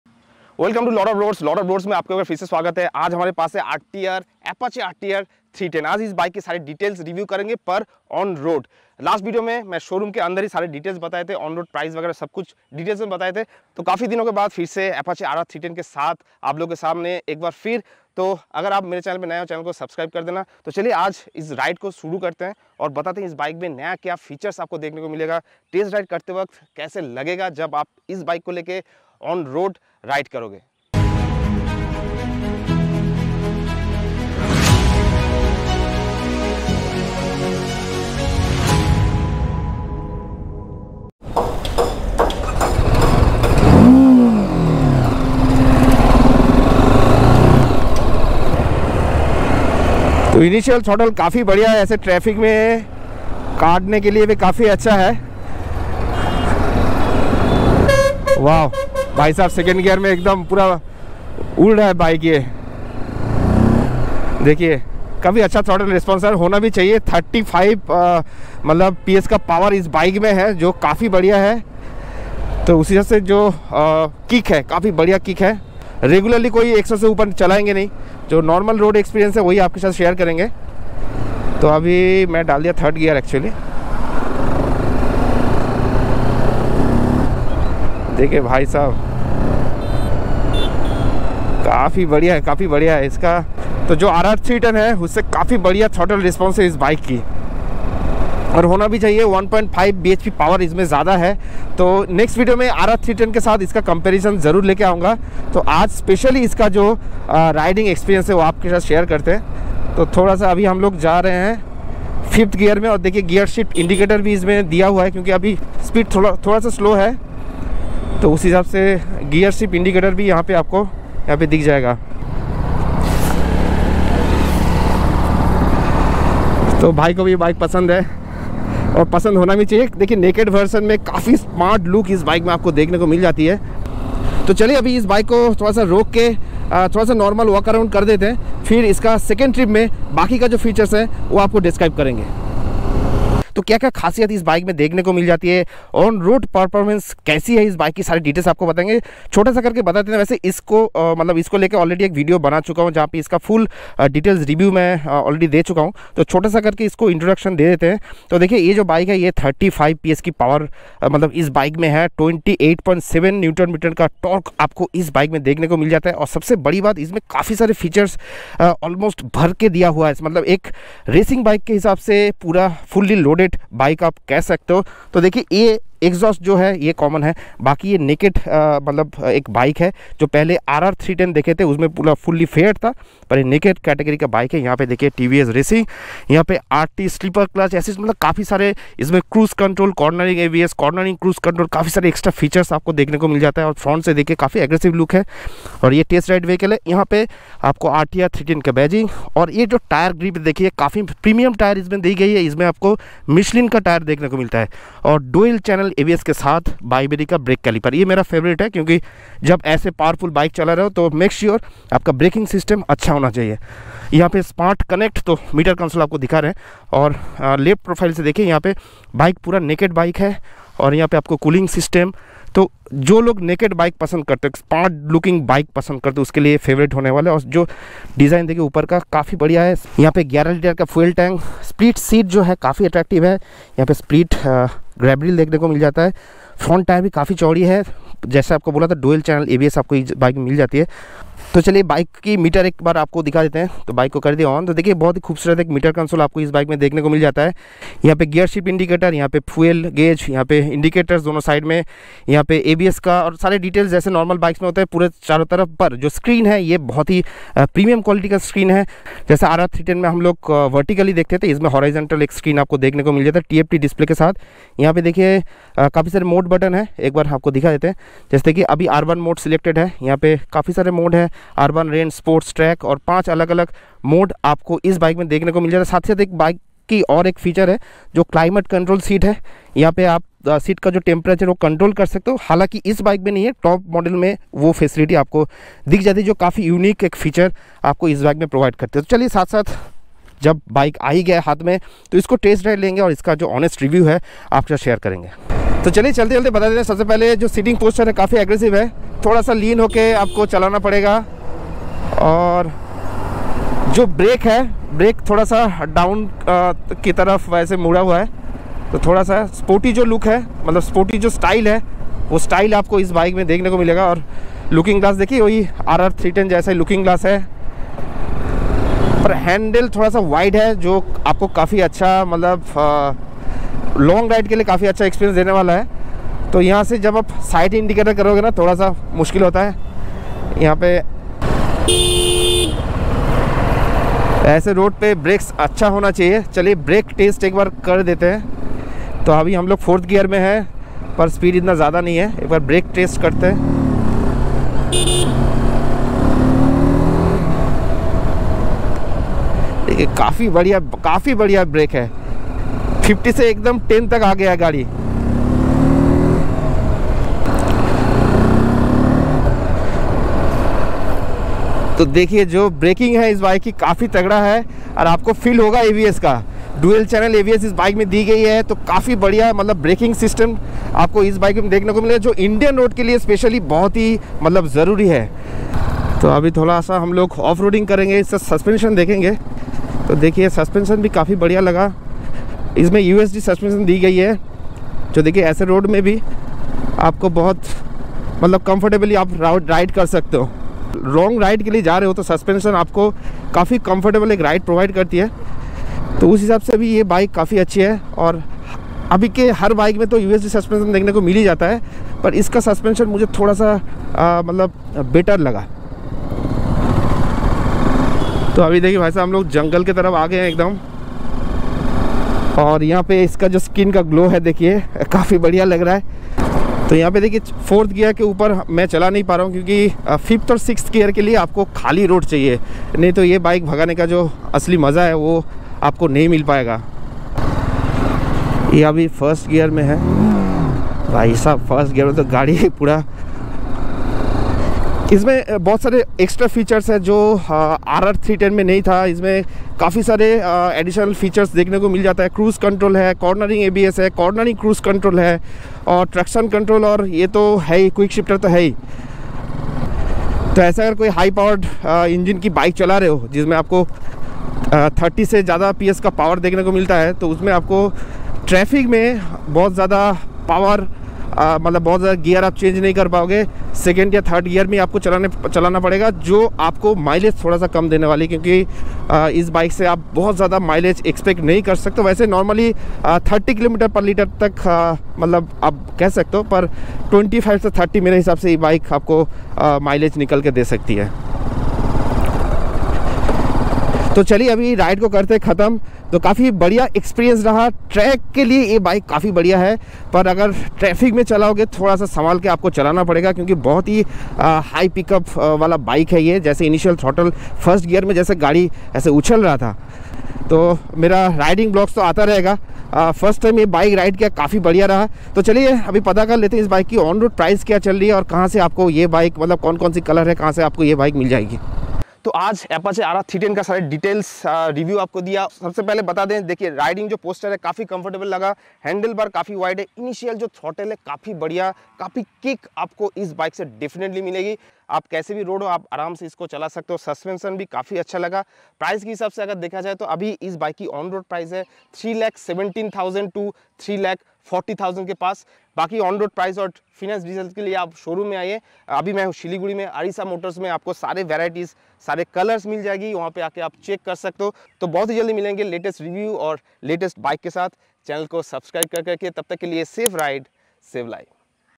TVS Apache RTR 310 Test Ride Review, Power Mileage On road Performance Test, with latest updates..
All new RTR310 with latest technology and racing features.